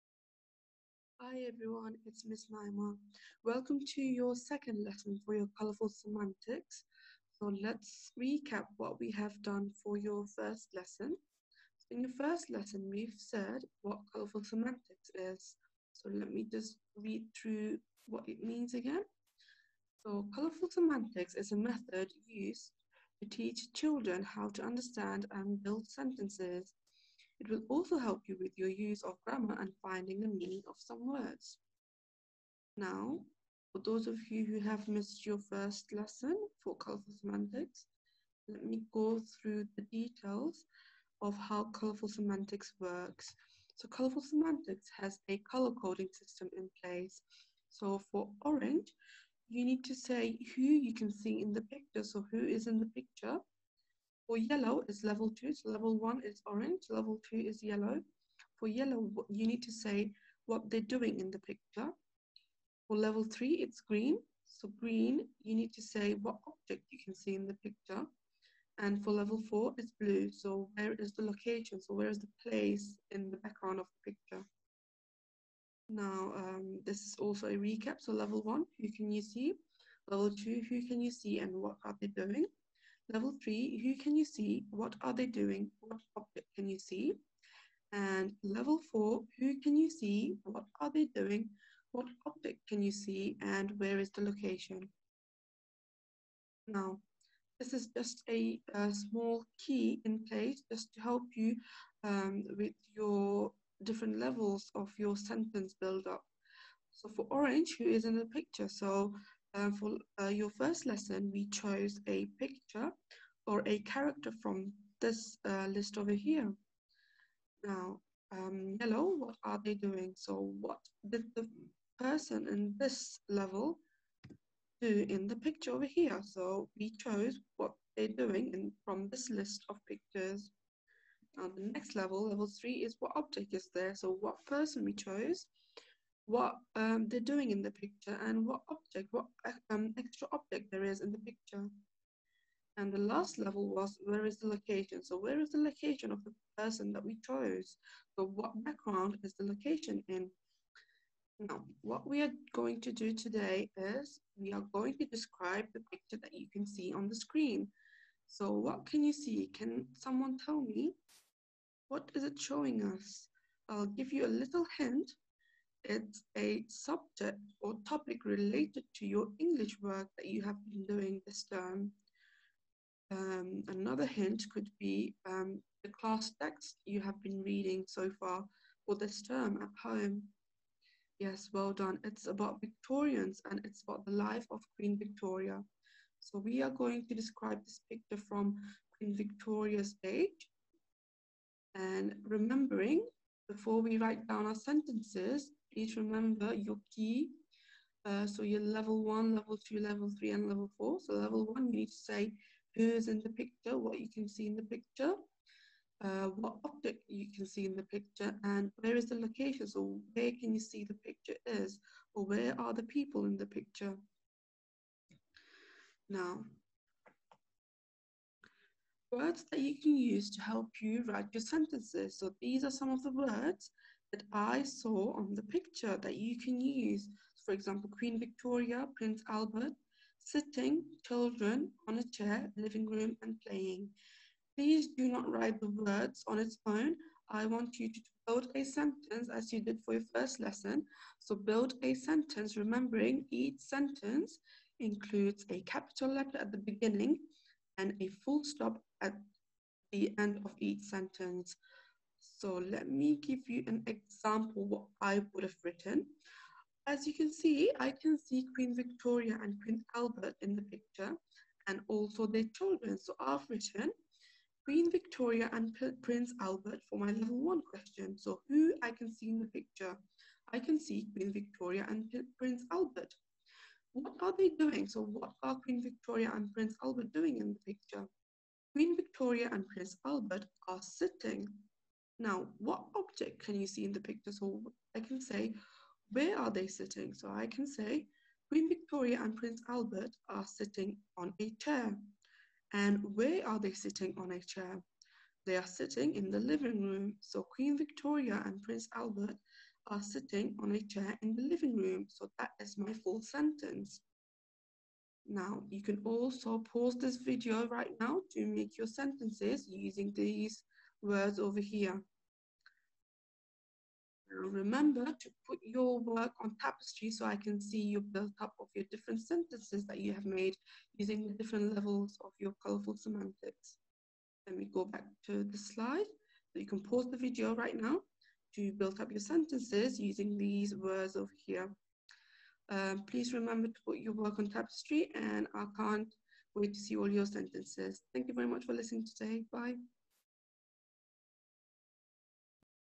Year Three - 14th January - Colourful semantics
Mrs Naima does some colourful semantics with you!